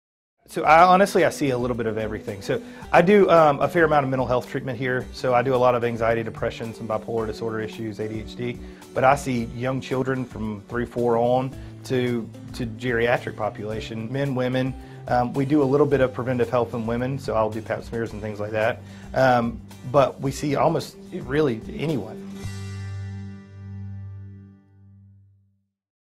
The patients I see - Baker Boles, MD - Inside Discover Health
Baker Boles, MD, sees a lot of different patients, but he works with a lot of people dealing with mental health issues (anxiety, depression and more).